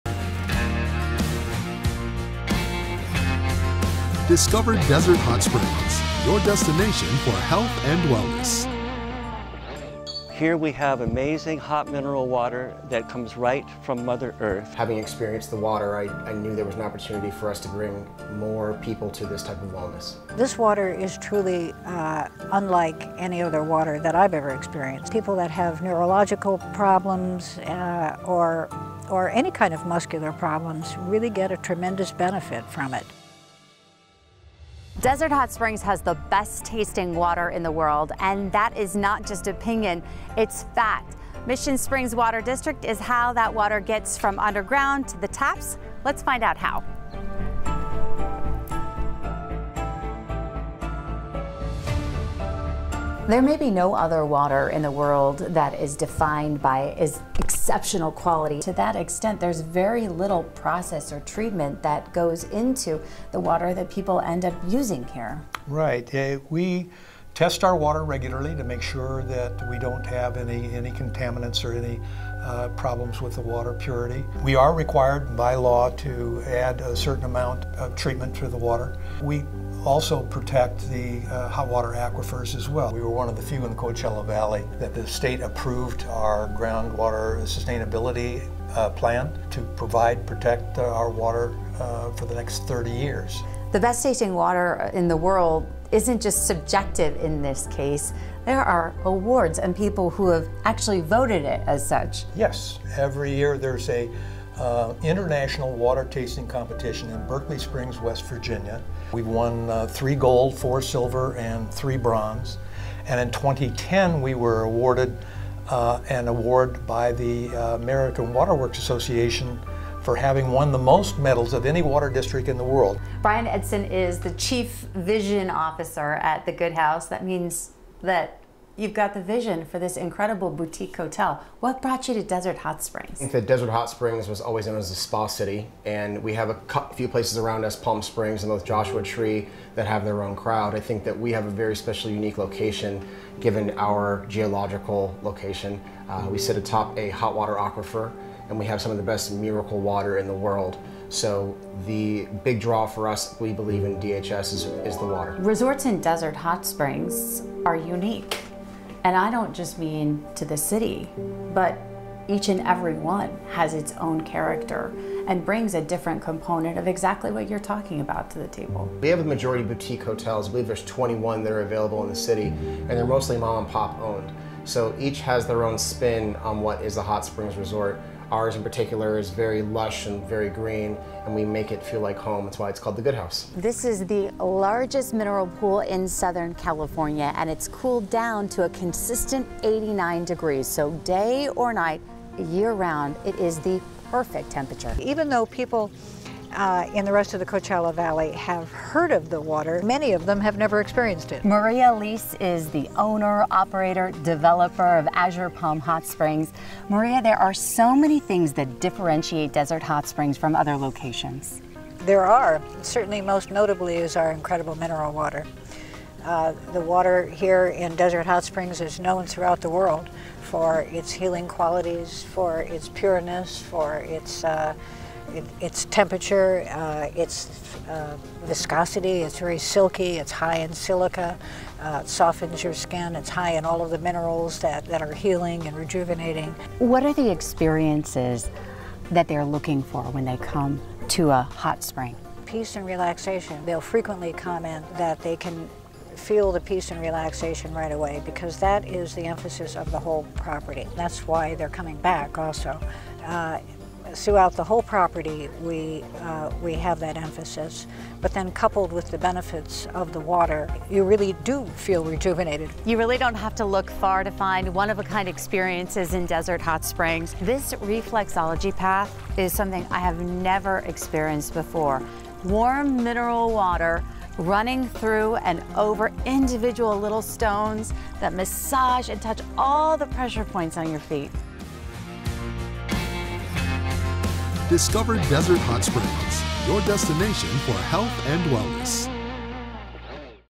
Desert Hot Springs Focus on Water with MSWD
Part of an NBC/DHS series on the uniqueness of Desert Hot Springs, this segment talks to both the hot and cold water aquifers under our feet.